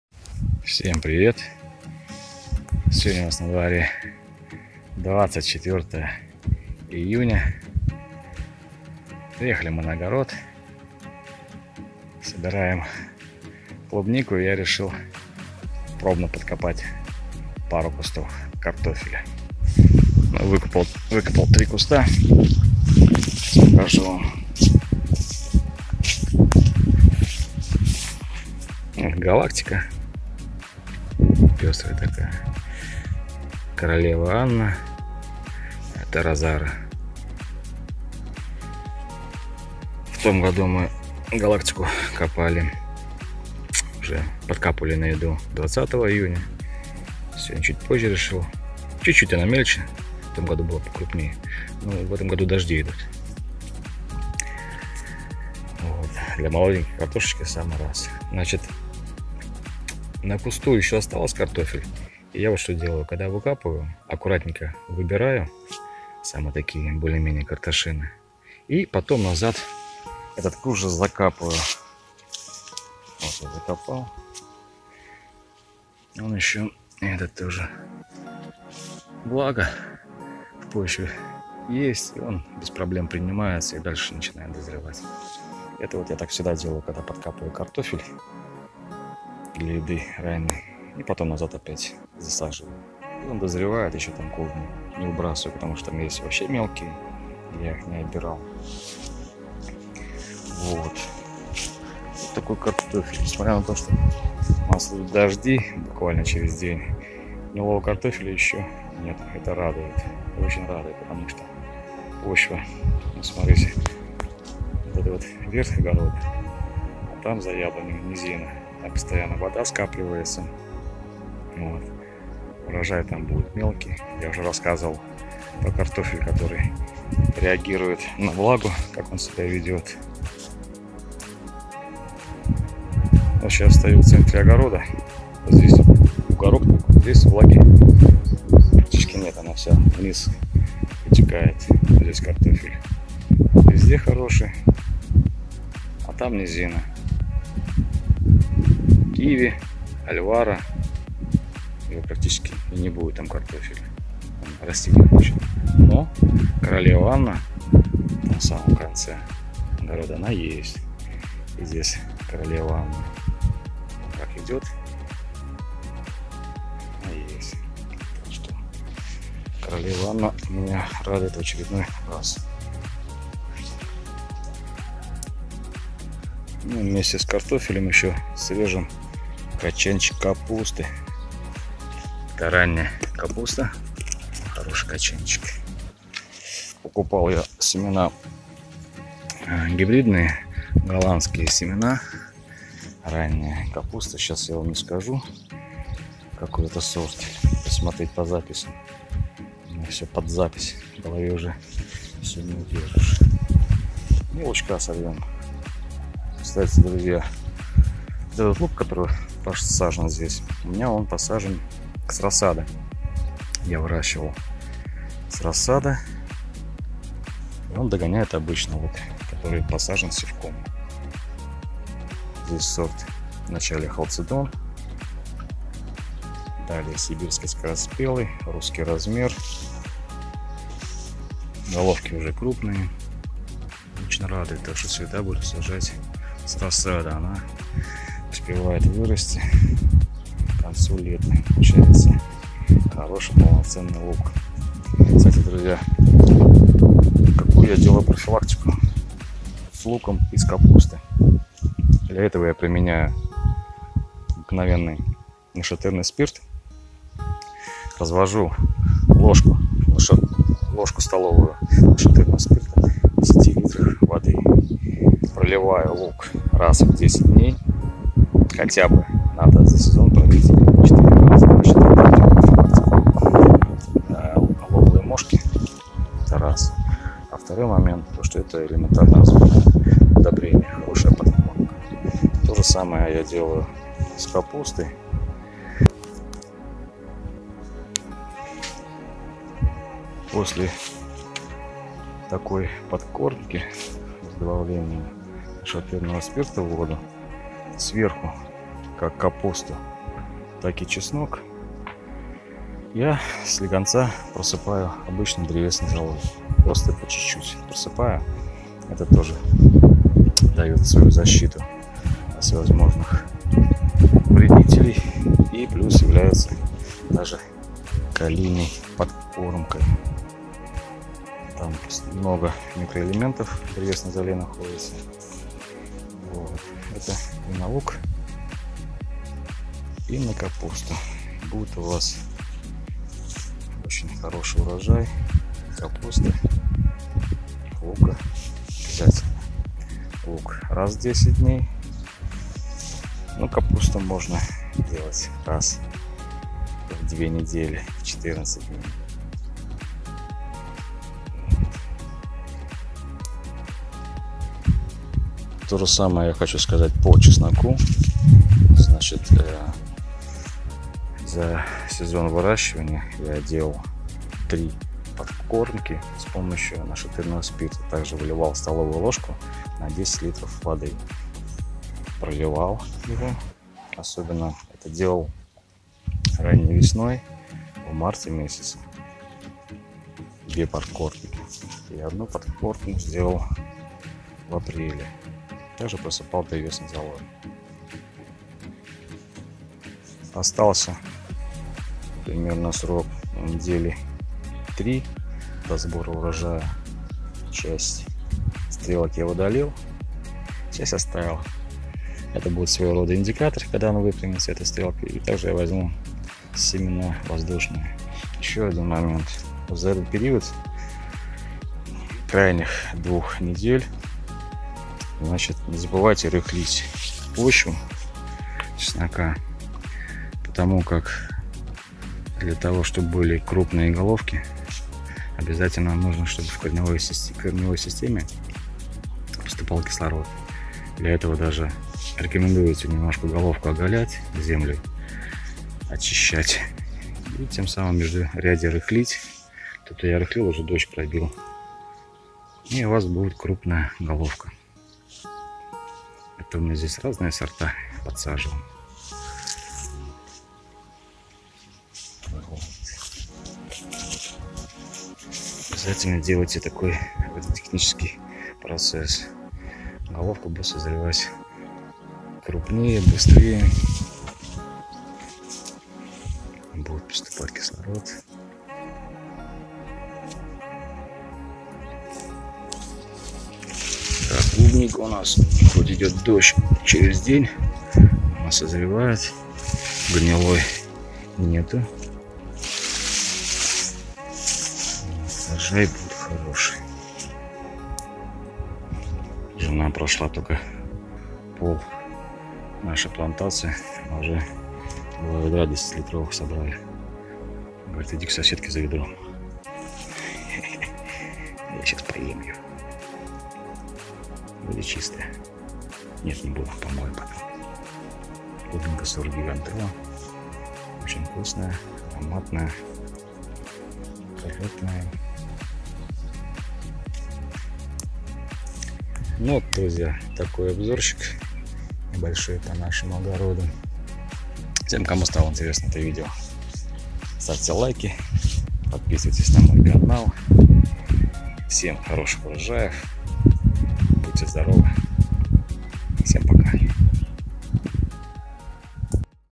Молодой картофеля в июне,ранняя капуста.Обзор огорода в июне/Успех в выращивании
Молодой катофель в июне,ранняя капуста/Обзор огорода в июне/Полезные советы  по выращиванию капусты и лука/Успех в выращивании